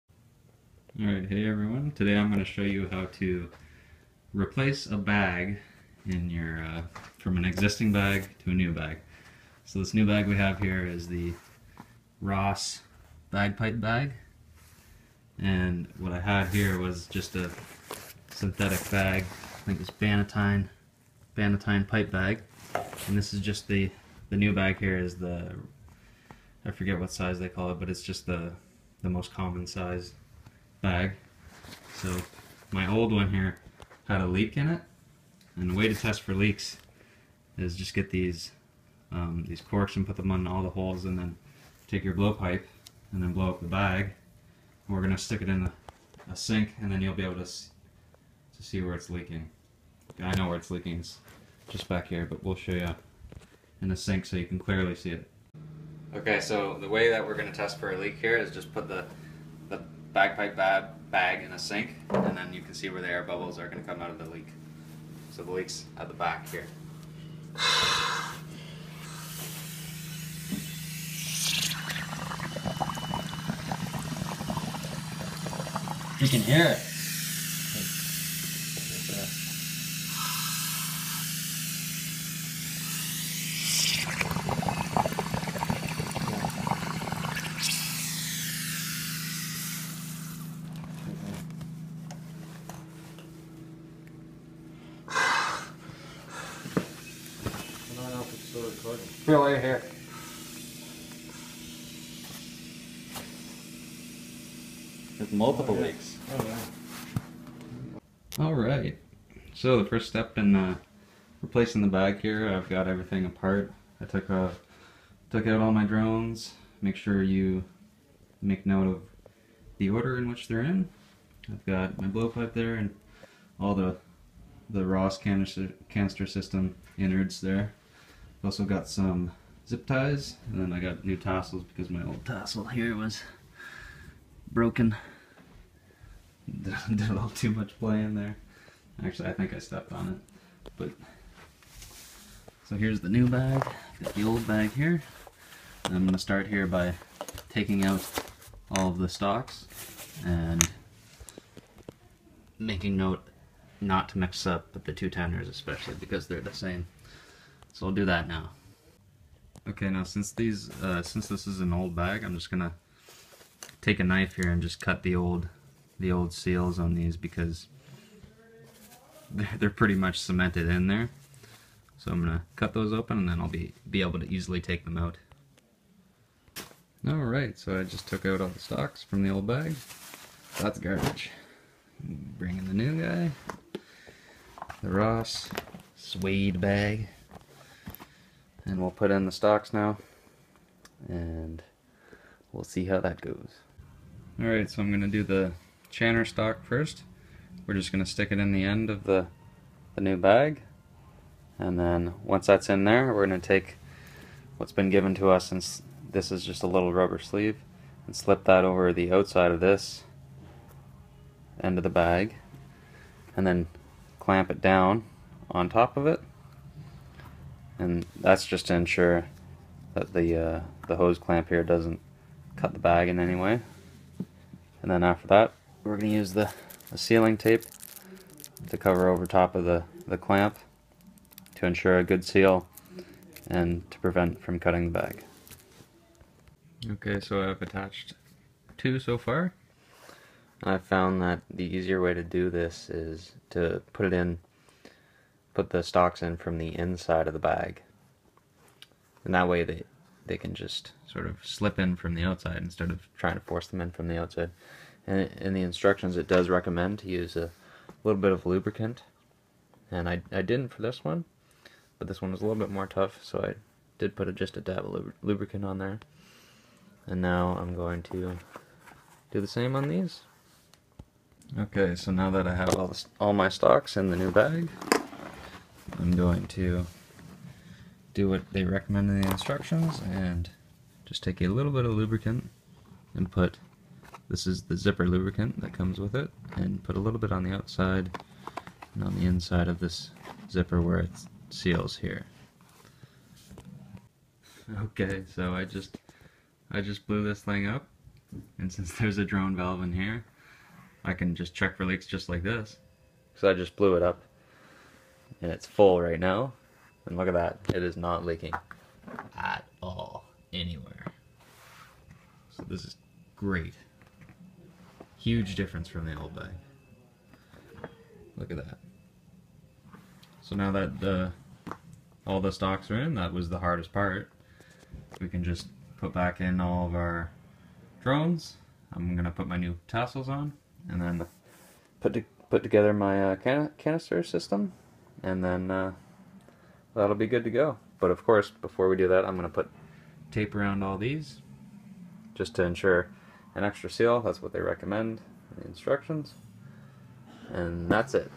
Ross Bagpipe Bag Replacement
Today I show you how a bad bagpipe bag leak can cause difficulty to play the bagpipes, and the solution to fixing that problem! I show you how to replace your old bag with the new ross bagpipe bag.